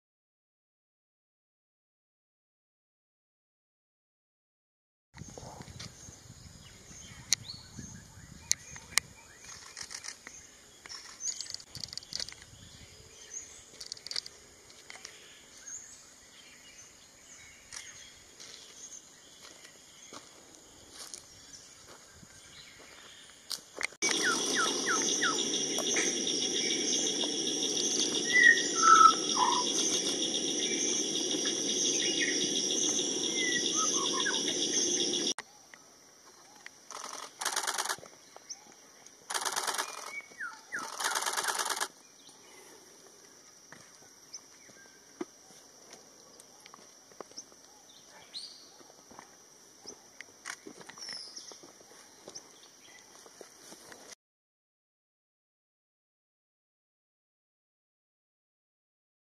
Andaman Shama
The Andaman shama is a species of bird in the family Muscicapidae. It's endemic to Andaman islands.